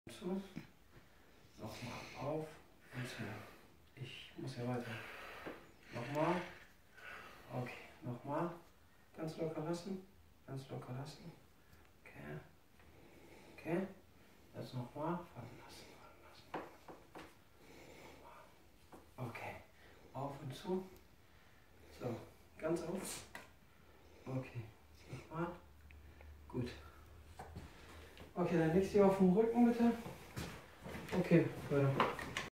Kiefer Behandlung von Sänger Mazlum
KIEFERSCHIEFSTAND
Von einem Kieferschiefstand spricht man, wenn die Bisslage und das Zusammenspiel von Unter- und Oberkiefer nicht mehr den individuellen physiologischen Gegebenheiten des Patienten entspricht.
Die Symptomatik ist vielfältig und wird häufig nicht direkt in einen ursächlichen Zusammenhang mit einem Kieferschiefstand gebracht.